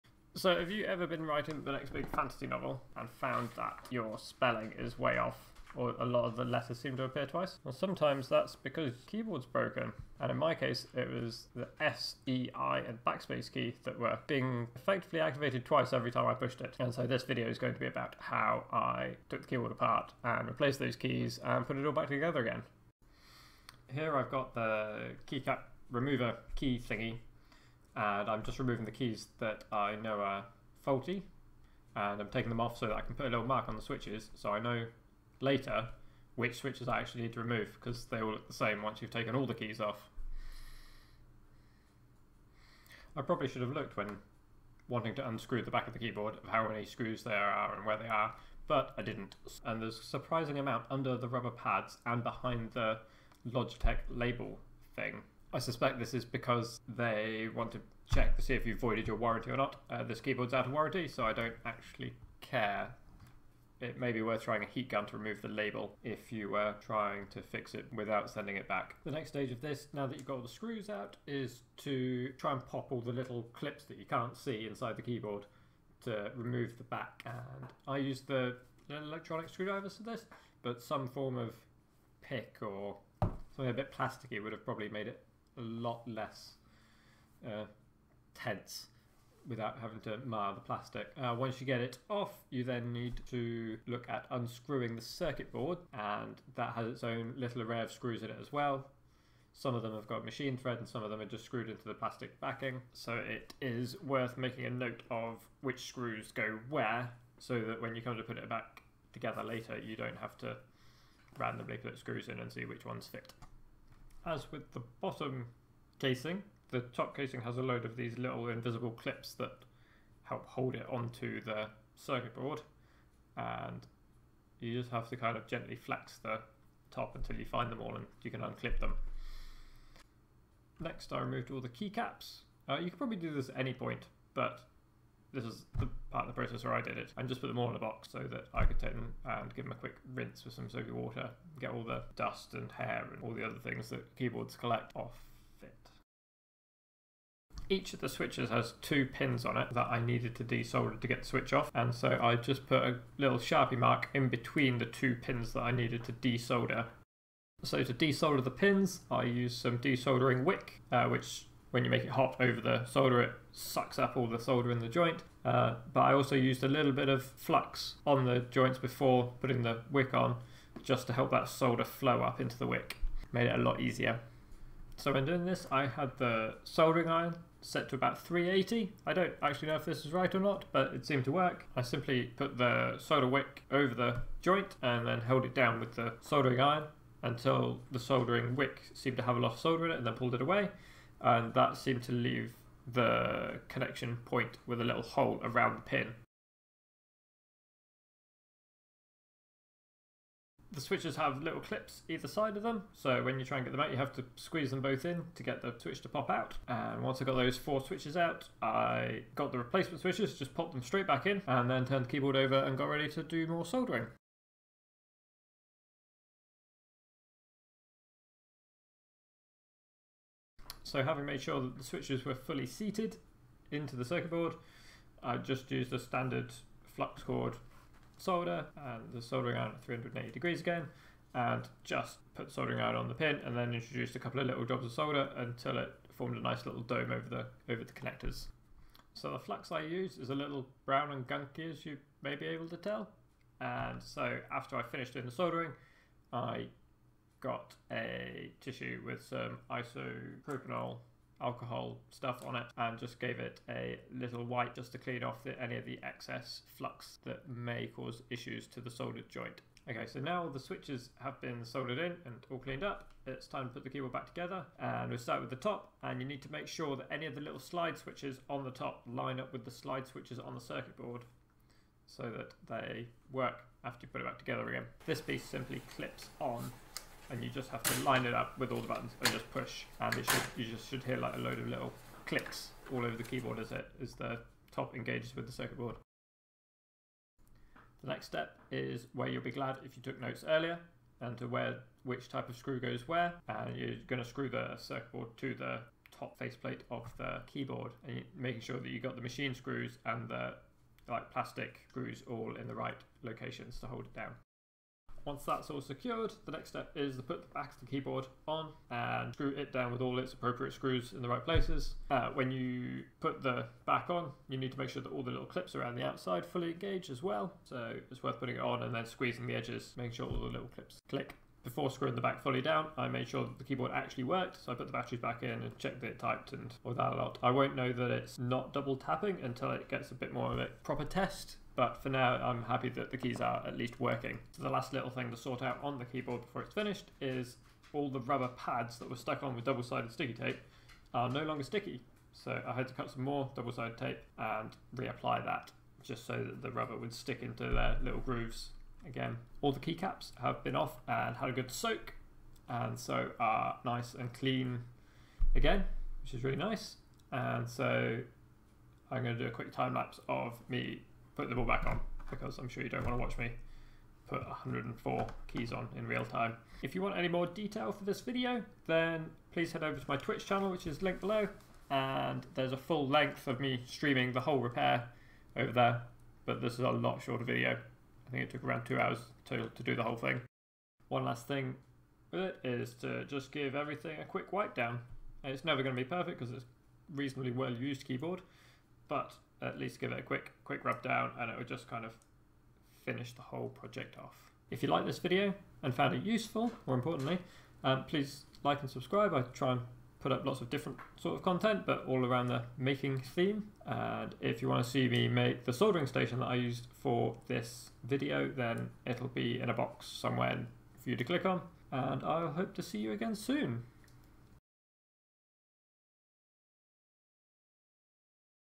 Keyboard Double Tap Issue, I Repaired my Logitech G613
So... I did a Twitch steam of the process of taking apart my G613 and replacing 4 key switches that were double tapping and then reassembling the keyboard.

I have since given the keyboard to my wife who is an author and spends most of the day typing and hasnt reported any issues back to me so let's call this one a win.

Just to be clear this is a mechanical keyboard that uses roamer G switches

These are afiliate links (just so you know).